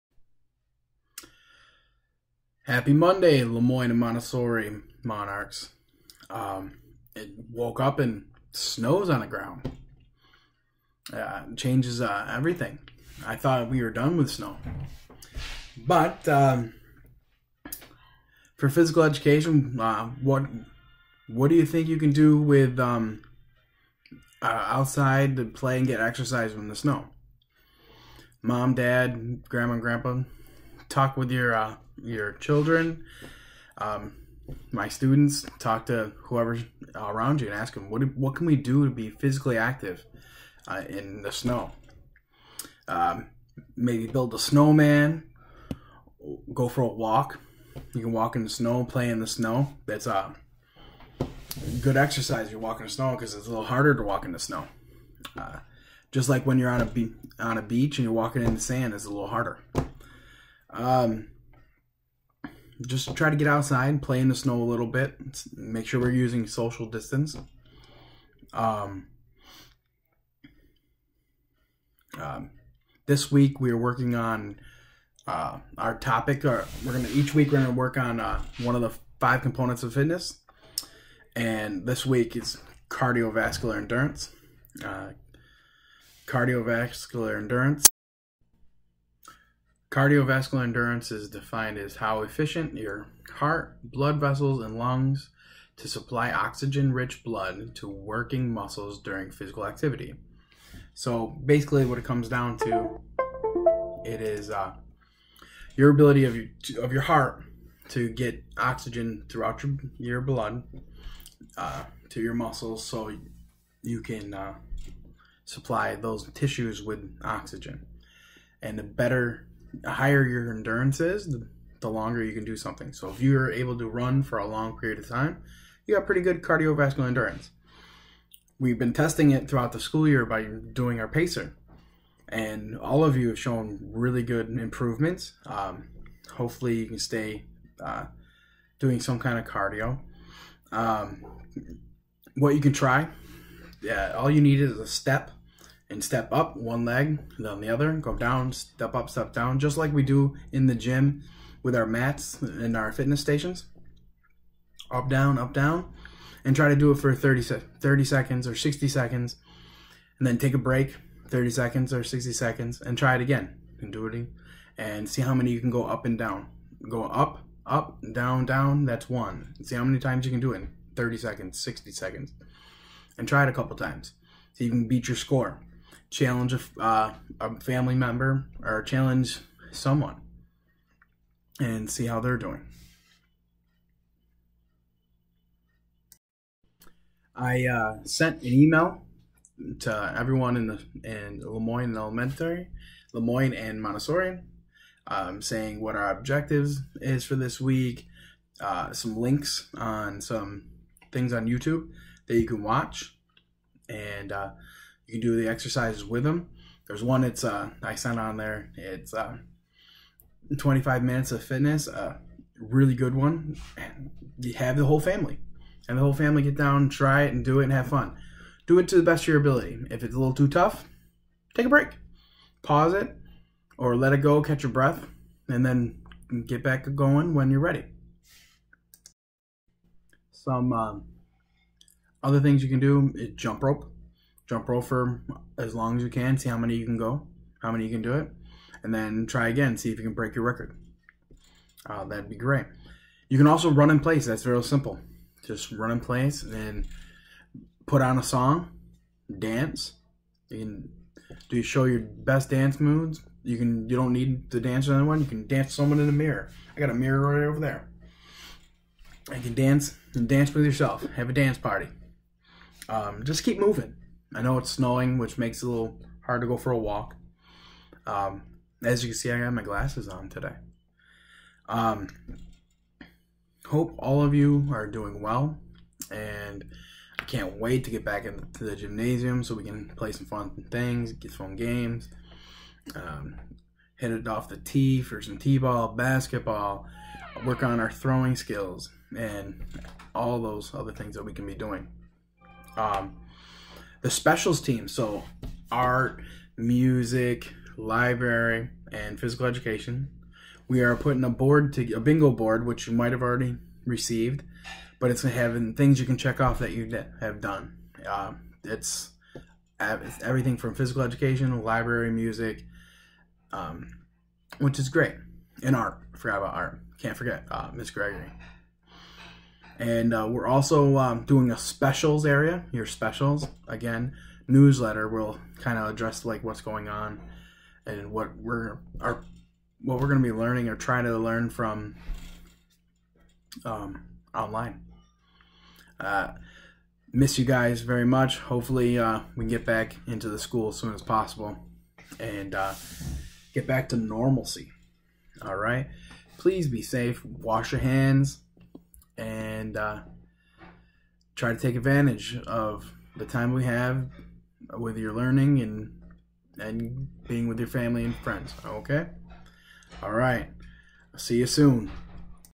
LeMoyne Elementary Physical Education 3/23/2020
Chat with Coach Stewart from Lemoyne Elementary.